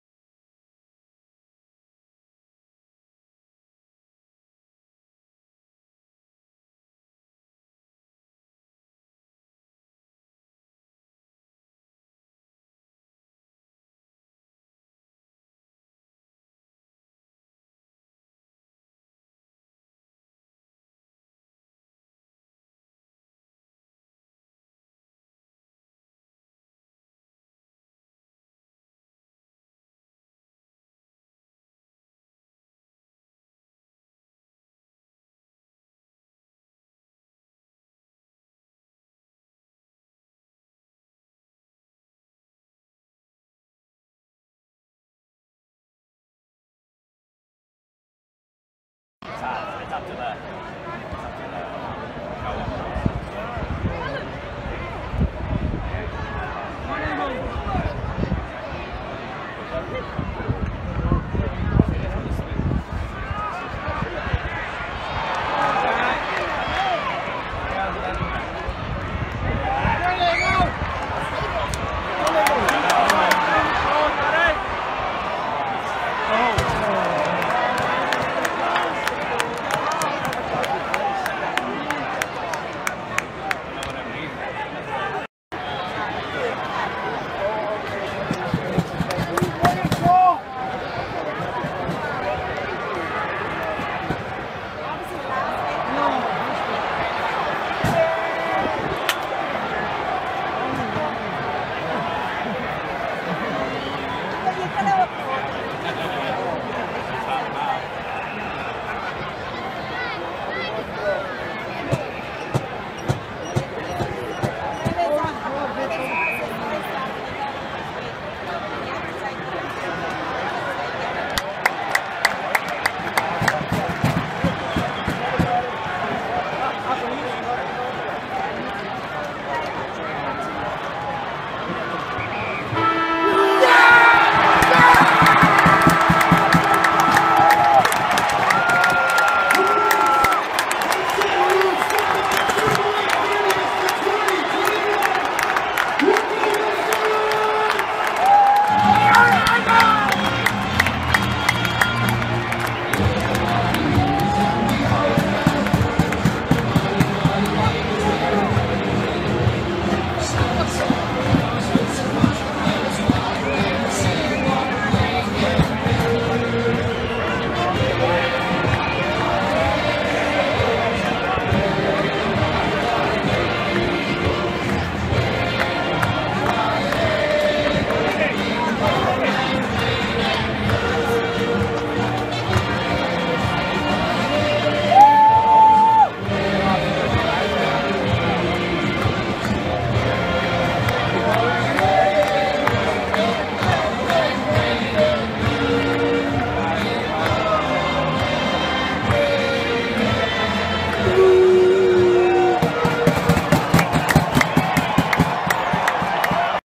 Photos and footage of the 2021 SANFL Grand Final
Woodville-West Torrens v Glenelg. My 15th Grand Final. Oct 3rd.